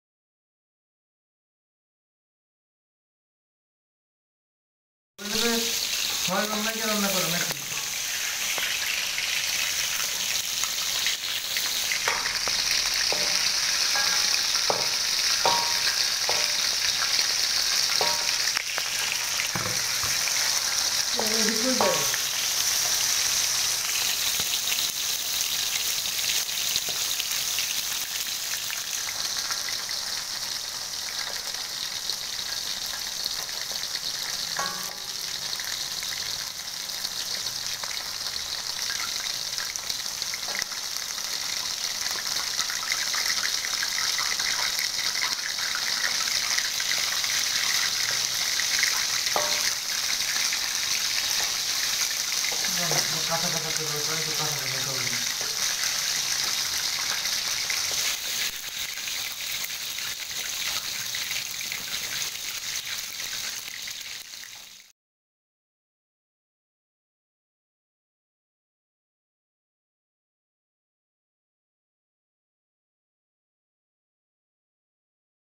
Malaysia, Recipe Checkes Fried, and Cooked Fish.
Malaysia's  Checks Curry Made,
Checks Fried Made,
Cooking Styles And Checks  Recipe.
 Fish Fried And   Fish Vaja.